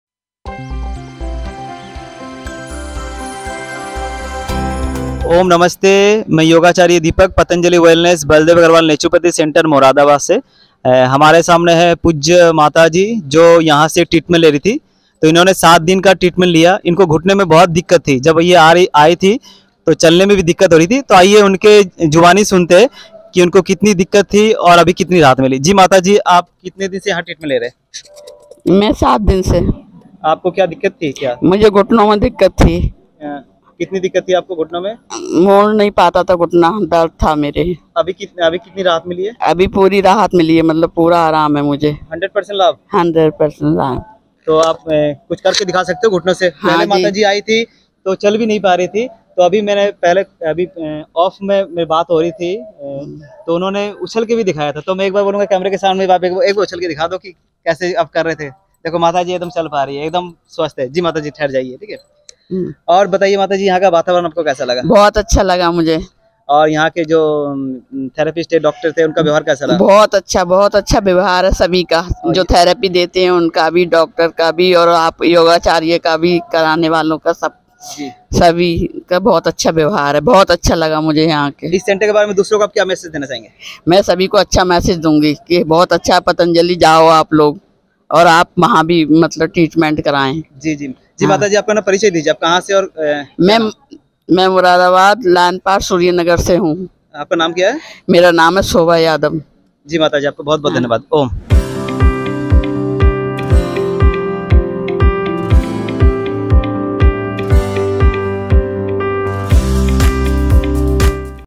मुरादाबाद से माताजी की सच्ची कहानी उन्हीं की जुबानी | स्वास्थ्य साधक B.A.N.C मुरादाबाद
पतंजलि वैलनेस बलदेव अग्रवाल Naturapathy सेंटर में घुटनों से परेशान माताजी ने ट्रीटमेंट कराया 7 दिन अब 100% आराम स्वोभा जी मुरादाबाद से